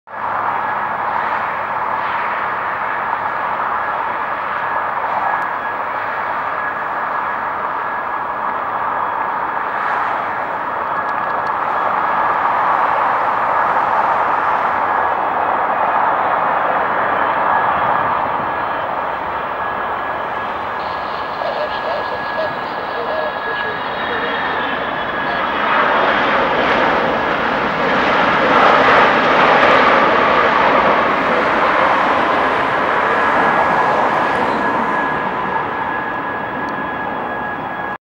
All Nippon Airways Boeing 777-300ER [JA789A] Landing at LAX.
Here is an All Nippon Airways Boeing 777-300ER landing at LAX runway 24R.

Boeing 777-381ER

*Nikon D5200 + Nikkor 70-300mm VR*